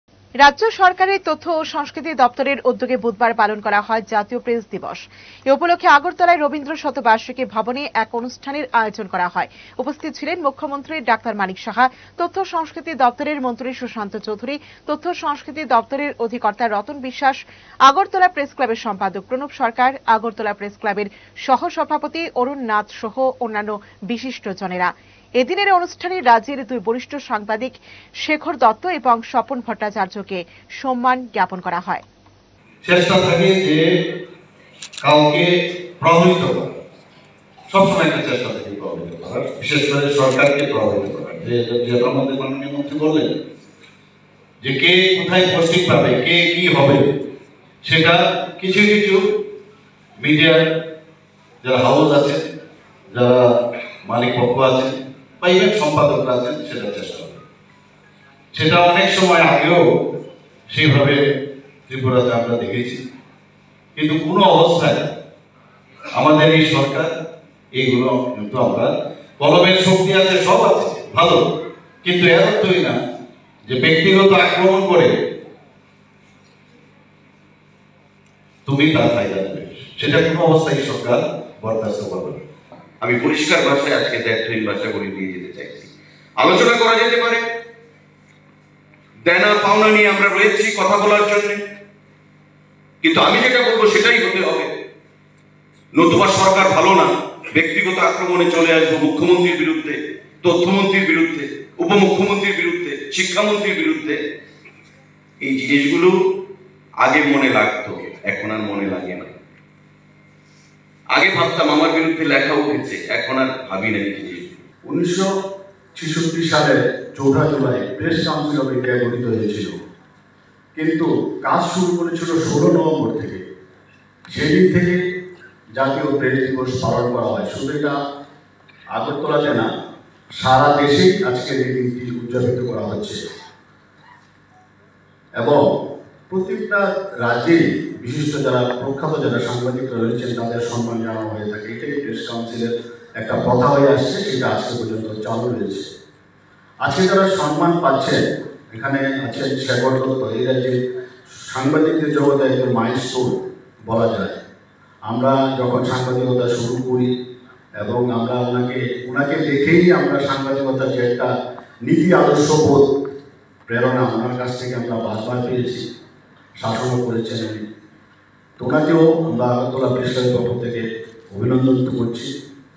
রাজ্য সরকারের তথ্য ও সংস্কৃতি দপ্তরের উদ্যোগে বুধবার পালন করা হয় জাতীয় প্রেস দিবস।
রাজ্য সরকারের তথ্য ও সংস্কৃতি দপ্তরের উদ্যোগে বুধবার পালন করা হয় জাতীয় প্রেস দিবস। এ উপলক্ষে আগরতলায় রবীন্দ্রশতবার্ষিকী ভবনে এক অনুষ্ঠানের আয়োজন করা হয়। উপস্থিত ছিলেন মুখ্যমন্ত্রী ডাক্তার মানিক সাহা ,তথ্য ও সংস্কৃতি দপ্তরের মন্ত্রী সুশান্ত চৌধুরী,   তথ্য ও সংস্কৃতি দপ্তরের অধিকর্তা রতন বিশ্বাস, আগরতলা প্রেসক্লাবের সম্পাদক প্রণব সরকার, আগরতলা প্রেসক্লাবের সহ-সভাপতি অরুণ নাথ  সহ অন্যান্য বিশিষ্টজনেরা। এদিনের অনুষ্ঠানে রাজ্যের দুই  বরিষ্ঠ সাংবাদিক শেখর দত্ত এবং স্বপন ভট্টাচার্যকে সম্মান জ্ঞাপন করা হয়।